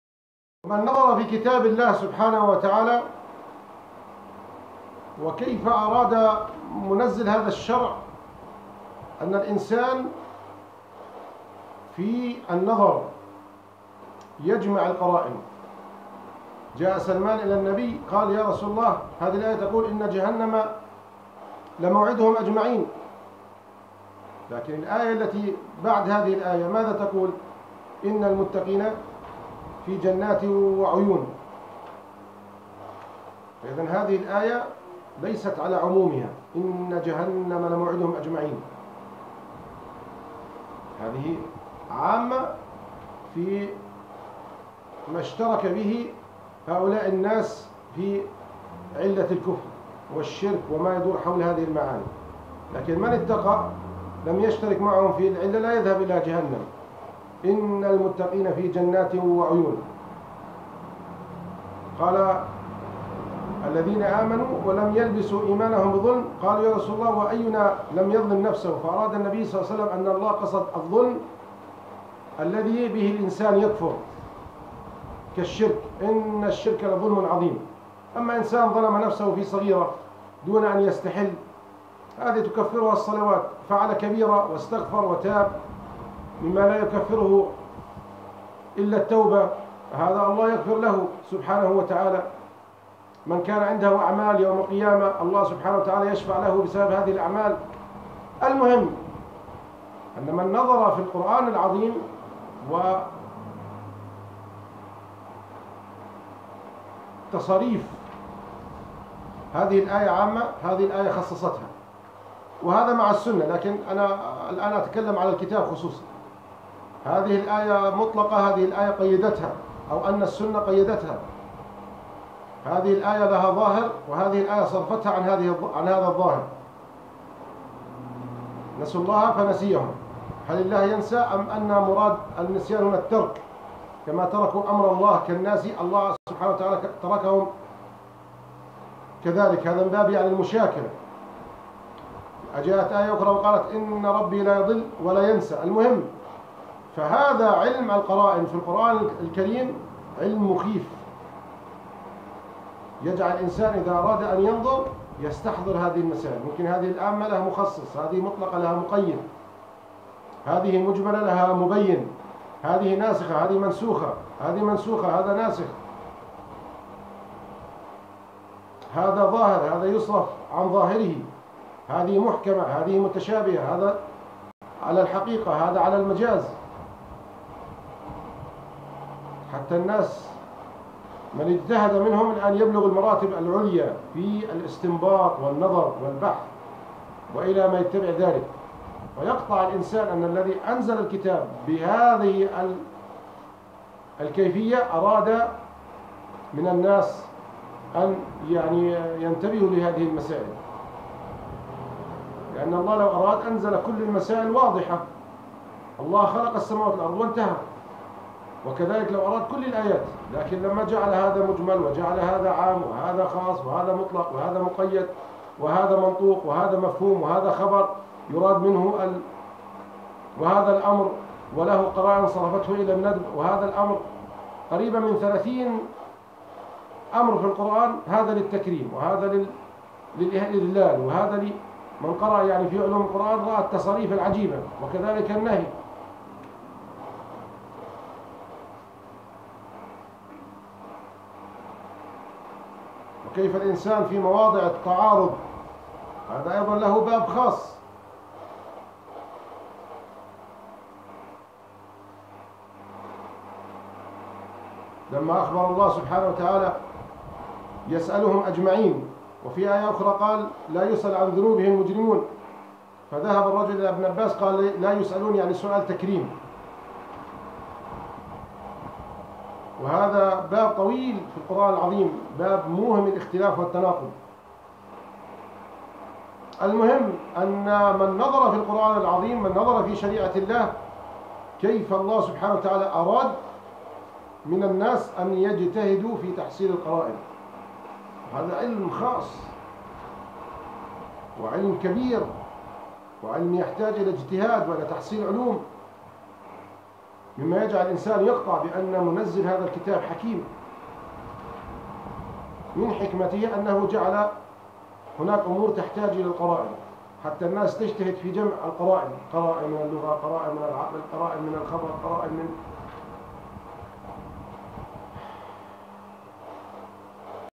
القرائن وإرادة الله البحث عنها في كتابه وسنته تدل على حكمته في فتح باب الإجتهادللناس وامتحانهم بالتق
قصص الأنبياء
باب حكمة الله